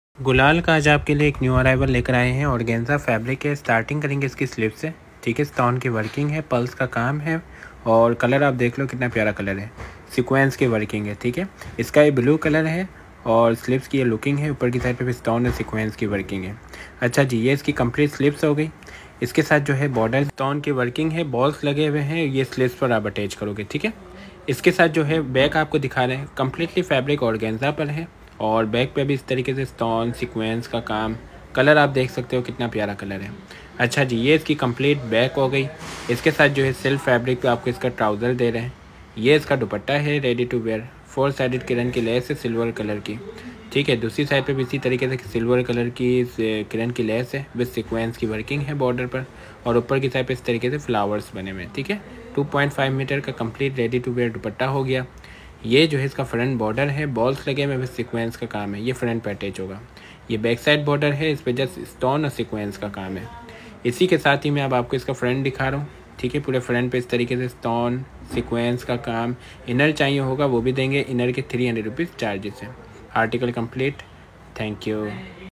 G-L Bridal Organza Price :5350/-| Ever Best Collection
G-L Bridal Organza
Price:5350/- Fixed with 250/- Delivery charges Apply Fixed Rate
Fabric Details
Shirt organza
Dupatta Organza Ready To Wear
Silk Trouser
Shirt lenght 45+
Shirt width 36.